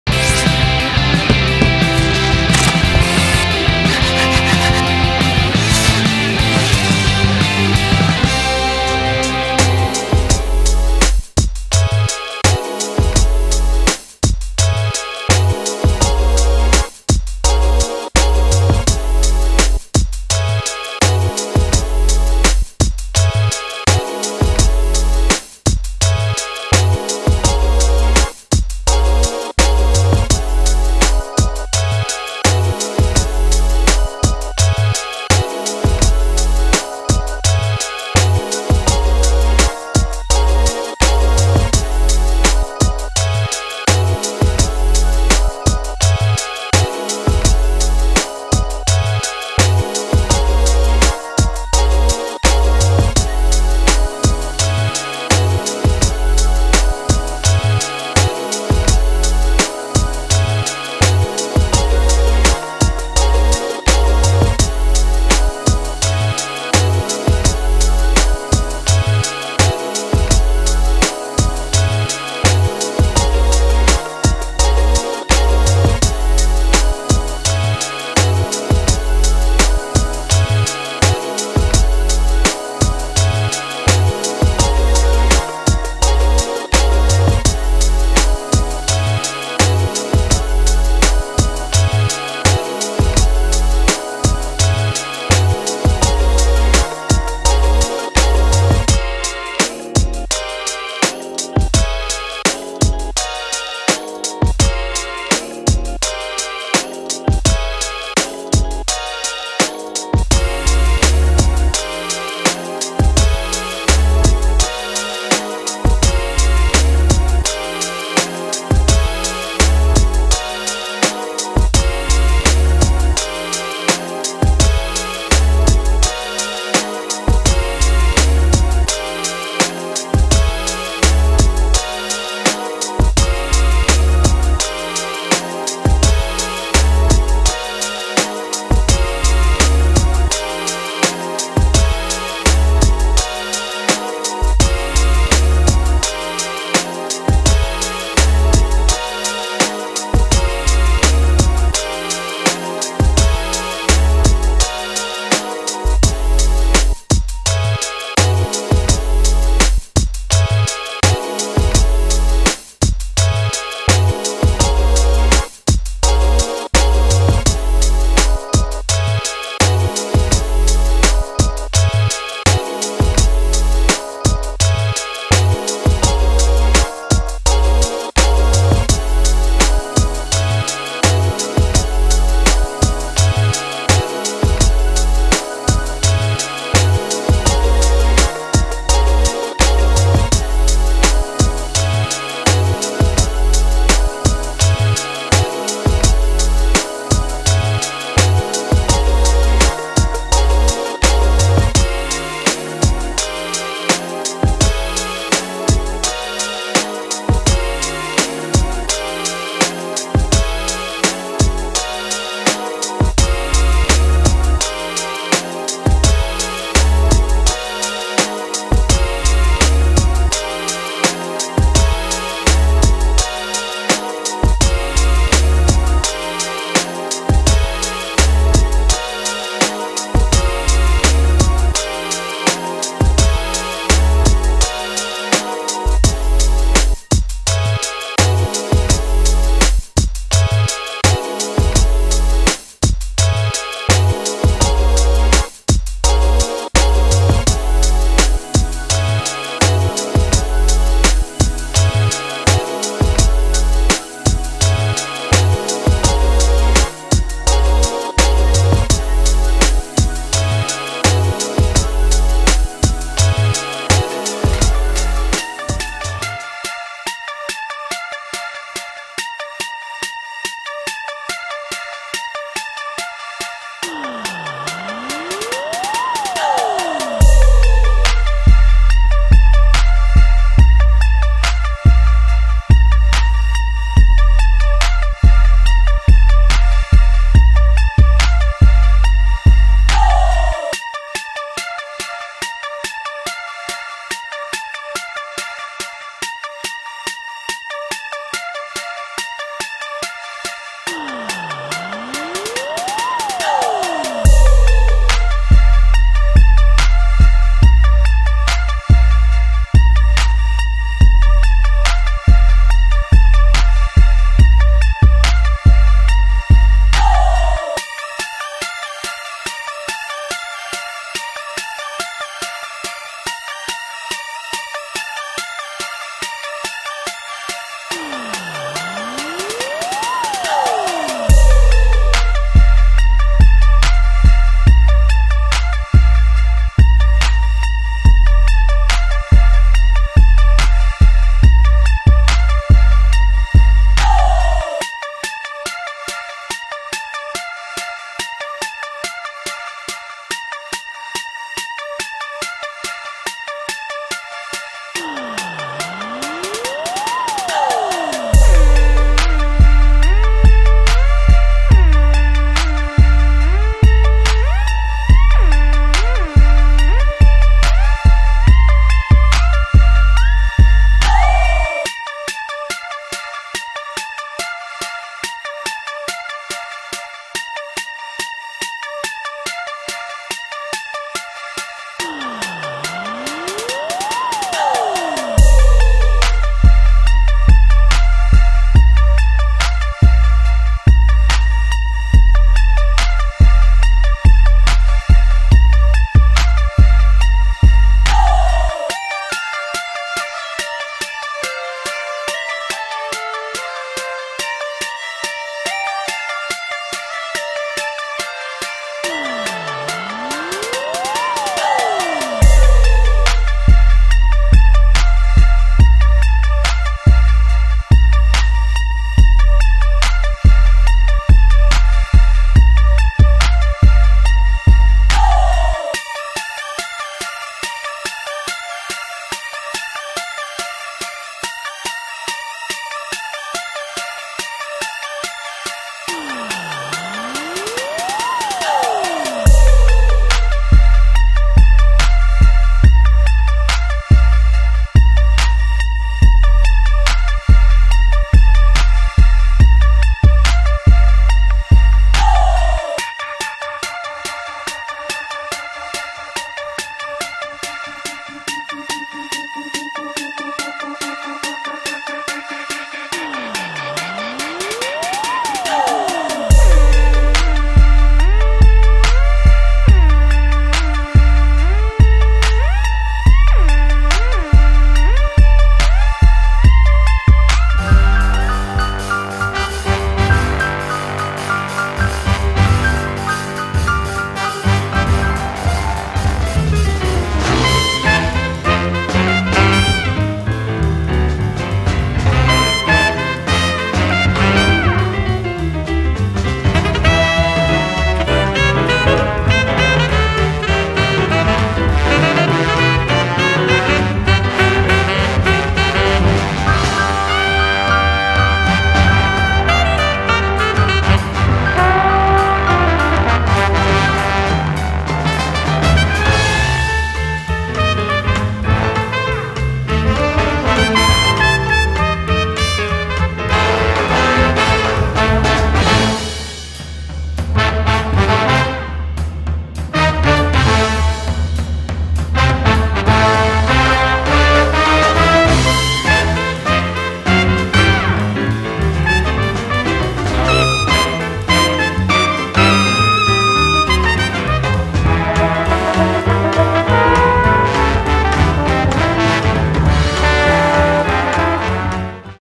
15 TRUQUES E ESPIÃO QUE TODA CRIANÇA OU ADULTO GOSTARIA DE SABER
Transforme-se em um espião de verdade com esses truques maravilhosos! Aprenda como fazer óculos de espião e esconda seus pertences onde ninguém jamais poderia encontrar! :)